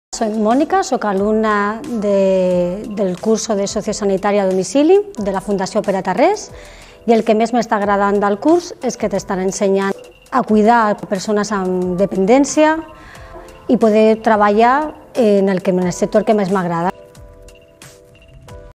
Certificat de professionalitat. Atenció sociosanitària a domicilis | Fundació Pere Tarrés
La Mónica, alumne del certificat de professionalitat d'Atenció sociosanitària a persones dependents a domicilis ens explica la seva experiència en aquesta formació.